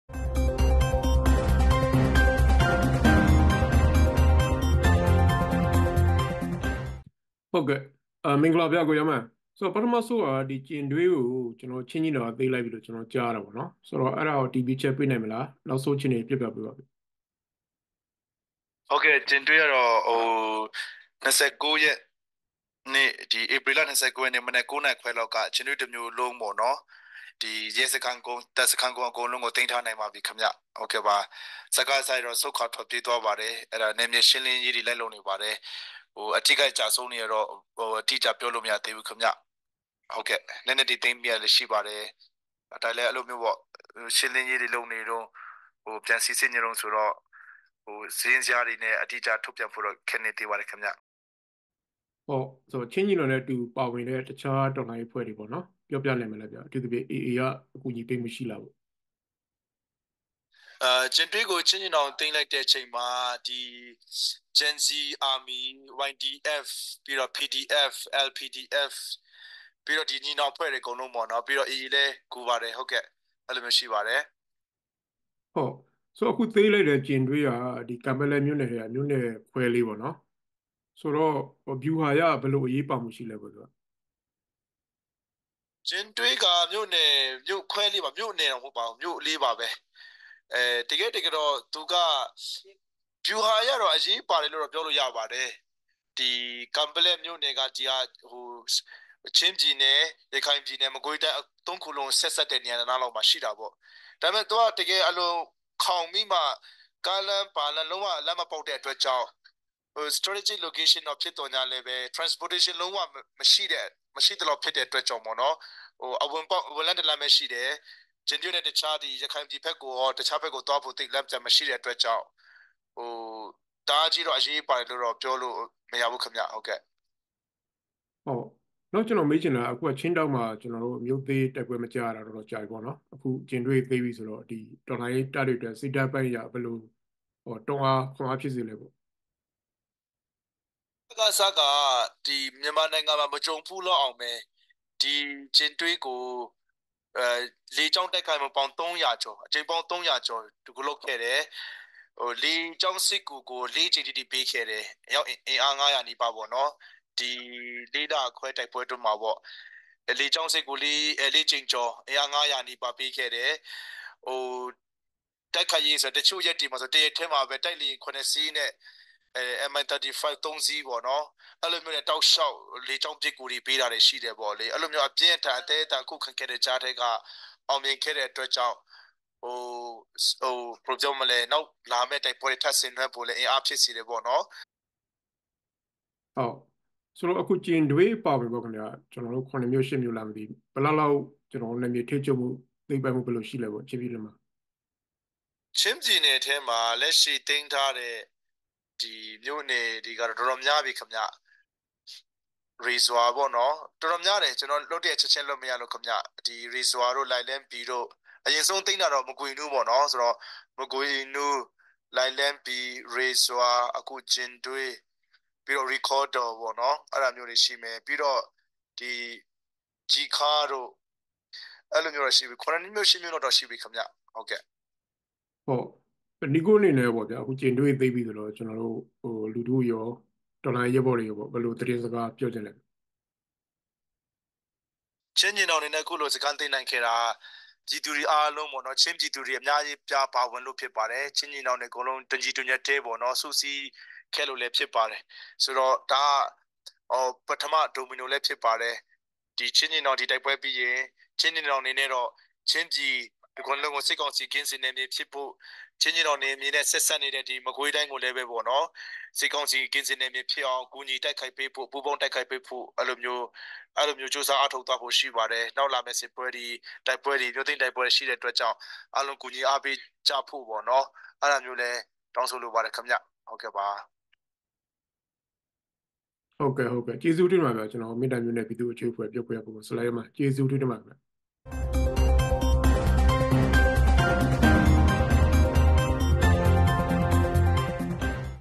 “ကျင်ဒွေးက ချင်းညီနောင်ရဲ့ ပထမ ဆုံး ဒိုမီနိုပါ”
- ဆလိုင်းယောမာန် (ပြောခွင့်ရပုဂ္ဂိုလ် - မင်းတင်မြို့နယ် ပြည်သူ့အုပ်ချုပ်ရေးအဖွဲ့)

“ကျင်ဒွေးဟာ မြိုငယ်လေးဆိုပေမယ့် ချင်းညီနောင်ရဲ့ ပထမ ဆုံး ဒိုမီနိုဖြစ်တယ်။ ချင်းပြည်နယ်မှာ စစ်ကောင်စီကင်းစင်တဲ့အထိ၊ မကွေးတိုင်းက ဆက်စပ်နယ်မြေတွေအထိ ဆက်လက်တိုက်ပွဲဖေါ်သွားမှာဖြစ်တယ်။ လူထုထောက်ခံမှုကြောင့် အခုလိုအောင်မြင်တာဖြစ်တယ်” လို့ ချင်းပြည်နယ် မင်းတပ်မြို့နယ် ပြည်သူ့အုပ်ချုပ်ရေးအဖွဲ့ ပြောခွင့်ရပုဂ္ဂိုလ် ဆလိုင်းယောမာန်က ပြောပါတယ်။

ချင်းပြည်နယ်၊ ကန်ပက်လက်မြို့နယ်အတွင်းရှိ ကျင်ဒွေးမြို့ကို ချင်းညီနောင်အဖွဲ့က ဧပြီ ၂၉ မှာ အပြီးသိမ်းလိုက်နိုင်တဲ့အခြေအနေ၊ ဗျုဟာအရ အရေးပါမှု အခြေအနေတွေနဲ့ ပတ်သက်ပြီး BNN သတင်းဌာနက ဦးထက်အောင်ကျော် မေးမြန်းစဉ် သူက အဲဒီလို ဖြေကြားခဲ့တာပါ။

အခု ထုတ်လွှင့်မယ့် မေးမြန်းချက်ထဲမှာ...

- ကျင်ဒွေးမြို့သိမ်းတိုက်ပွဲ နောက်ဆုံးအခြေအနေ...

- ဒီမြို့က ဗျုဟာအရ ဘယ်လိုအရေးပါမှုရှိသလဲ...

- ကျင်ဒွေးဟာ ပထမ ဆုံး ဒိုမီနို လို့ ဘာကြောင့်ပြောနိုင်တာလဲ...

... စတဲ့အကြောင်းအရာတွေကို ဆွေးနွေးတင်ပြထားပါတယ်။